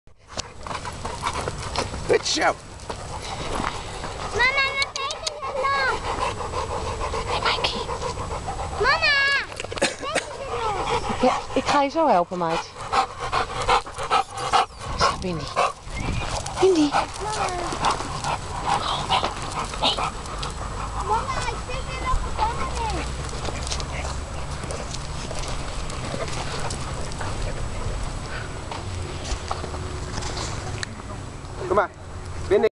Bullmastiffs Mike en Bindi in het bos
Mike 8 months (fawn), Bindi 2 years (red)